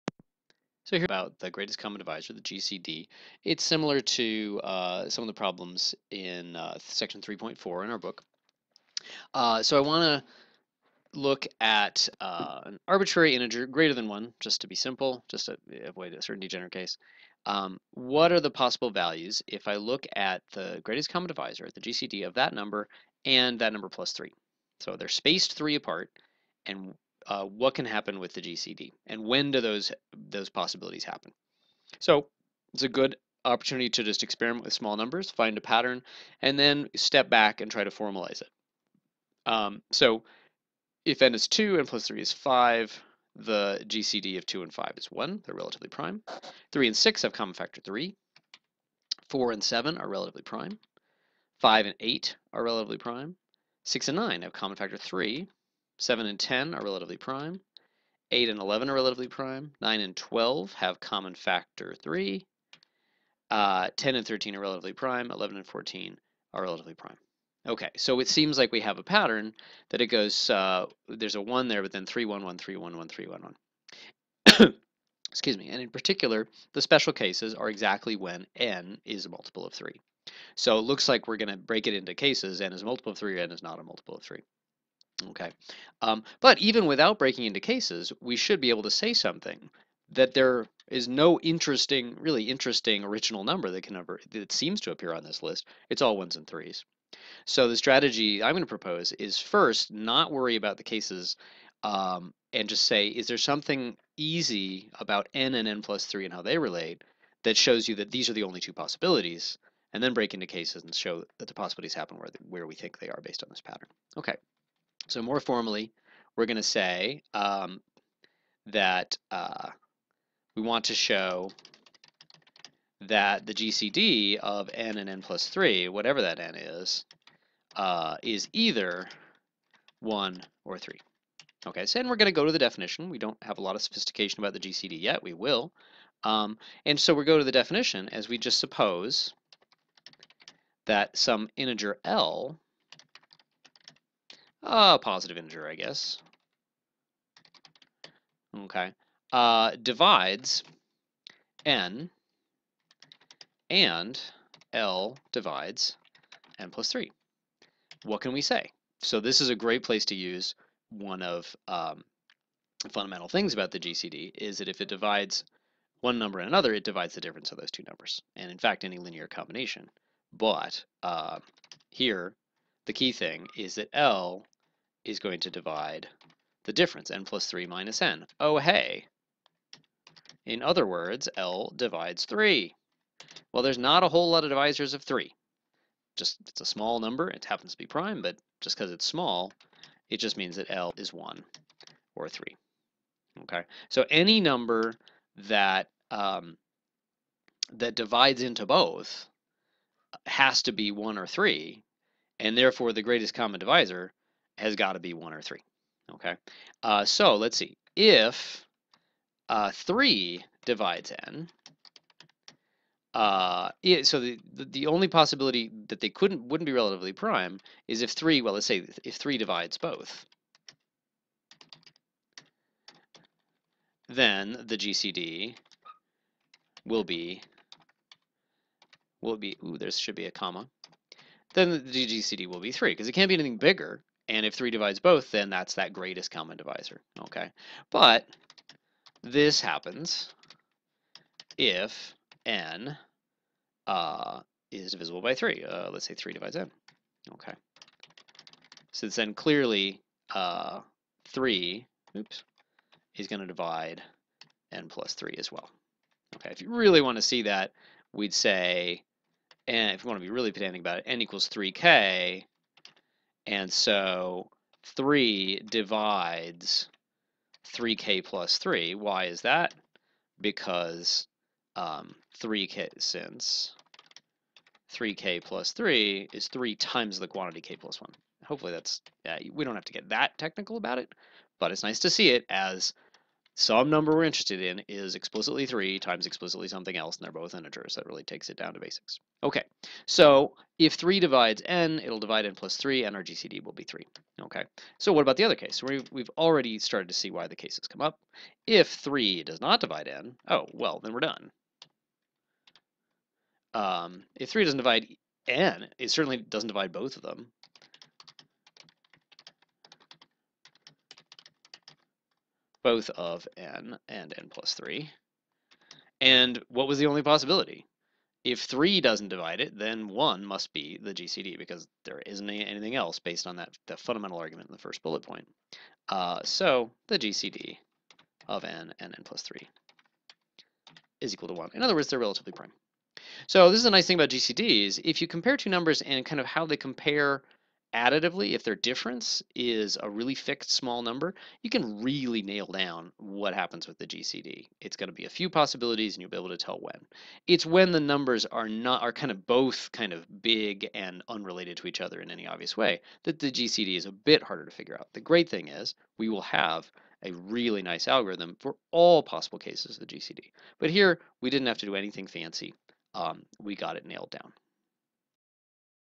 A problem on the GCD
A problem asking what the gcd(n,n+3) can be. A good introduction to general problems about the gcd (greatest common divisor).